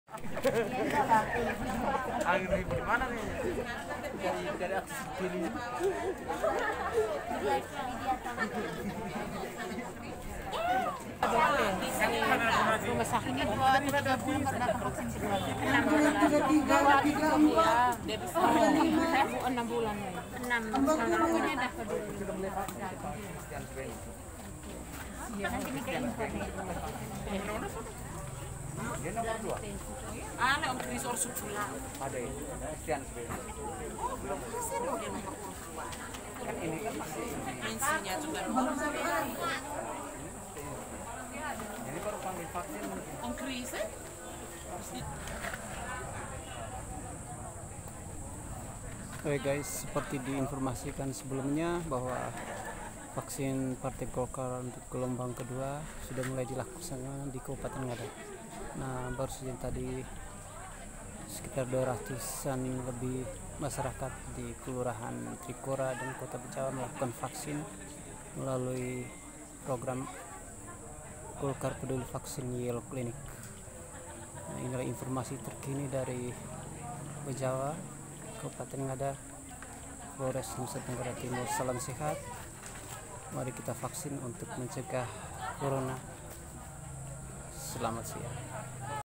Vaksin Yellow Clinic Gelombang 2 Dosis 1 kembali di gelar hari ini, Lokasi Gedung Golkar Kab Ngada
Pantauan MPBF Vaksin Gelombang ke 2 Dosis 1 untuk astra diberikan kepada masyarakat Kab Ngada, Tujuan untuk menekan perkembangan Covid di Wilayah NTT umumnya dan Kab Ngada Khususnya.
untuk masyarakat yg belum vaksin untuk melakukan vaksin hari ini di lokasi ini lebih dr 200 orang menerima vaksin di Kota Bajawa.